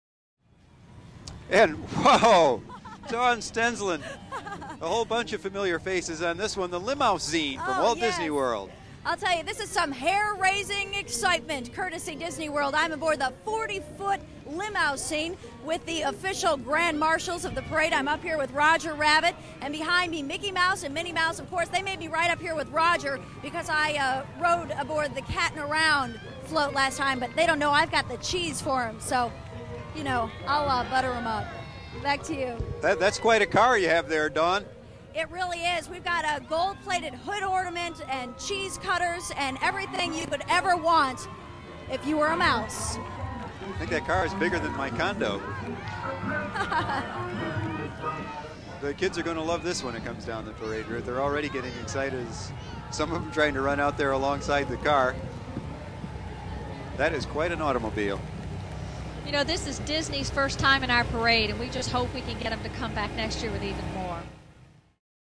Mickey, Minnie and Roger Rabbit in The 1991 Edison Festival of lights Parade
Here's some Rare footage of Mickey Mouse, Minnie Mouse and Roger Rabbit in The 1991 Edison Festival of lights parade riding in the Disney MGM Studios's LiMOUSEsine.